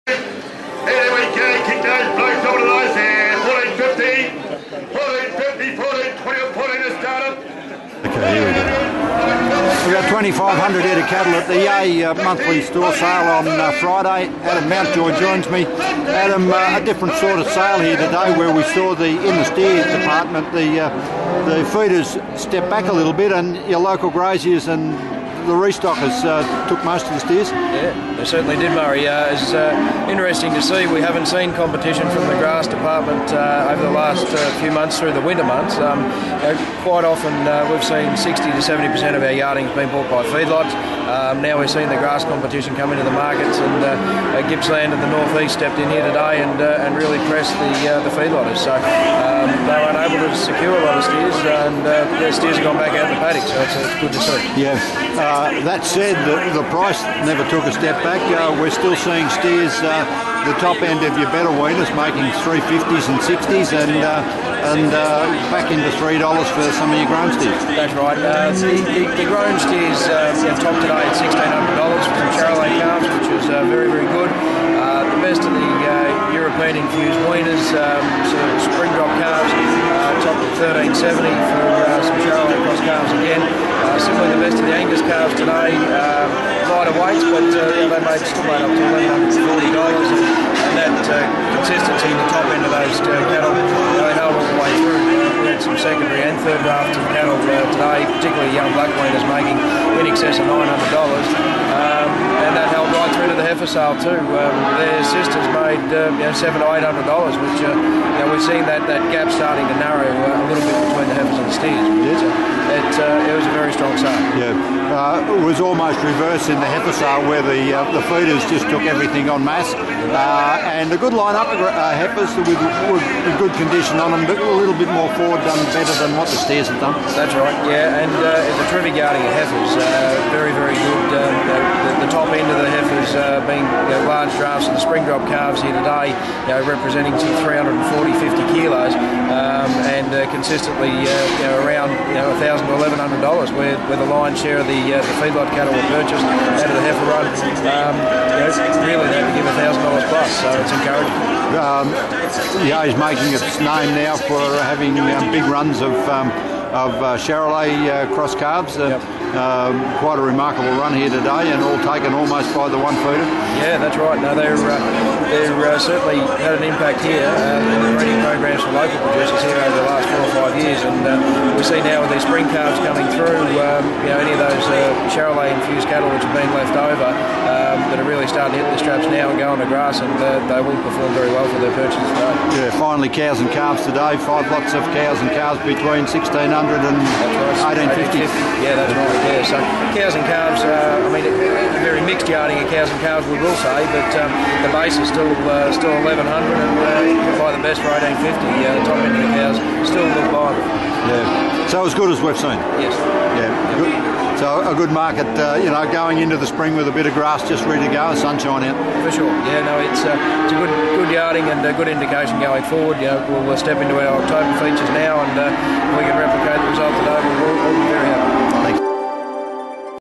Grass finishers pressure Yea steers
Northeast and Gippsland restockers increased the pressure on the store steer market at Yea saleyards on Friday pushing aside a solid body of feeder buyers which by the end of the day dominated sales in the unjoined heifer market.
Prices paid for grown steers varied between $1350 and $1600 a head while lighter weaners made $850- $1270. The gap in steer and heifer prices was also seen to narrow as heavier weights of heifers suitable to feed made $950-$1100 while lighter heifers $740- $900 in a full penning that offered 2600 head overall.